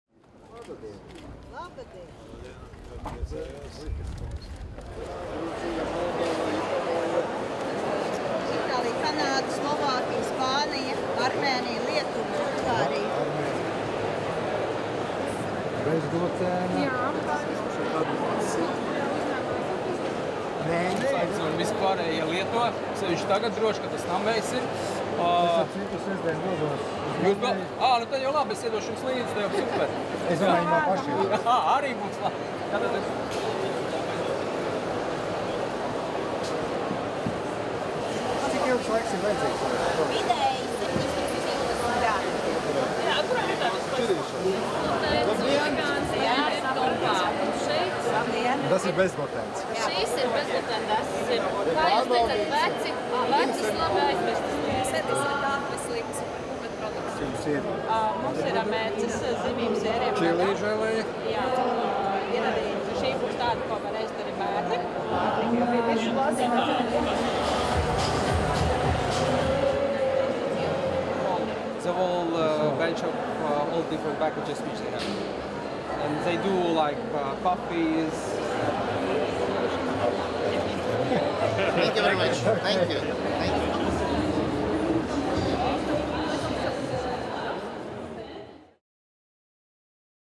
Izstādes "RigaFood 2024" apmeklējums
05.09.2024. Izstādes "RigaFood 2024" apmeklējums.
Video autors: Gatis Bergmanis, Valsts prezidenta kanceleja.